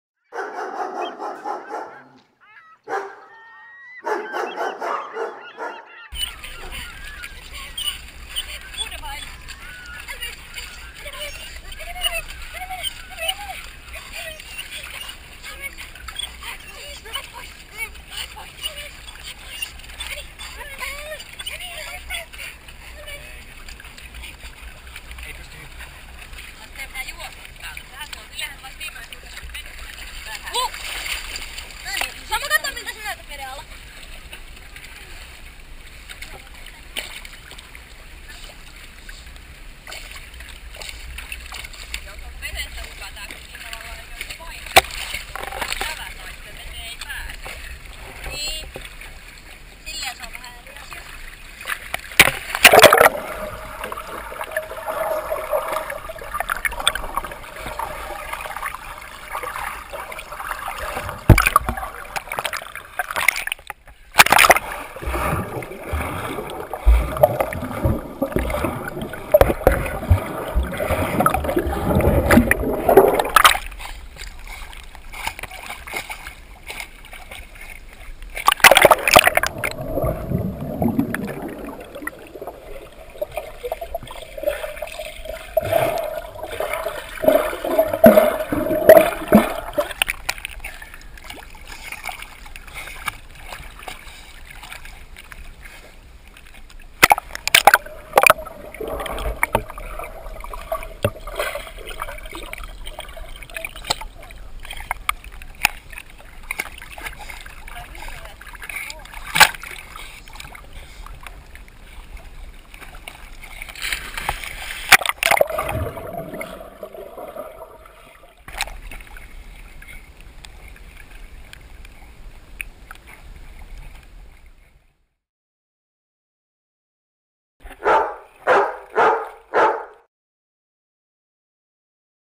Elvis the dog uimassa
Elvis uimassa Haukivedellä.
(kuvattu Go Pro HD -kameralla)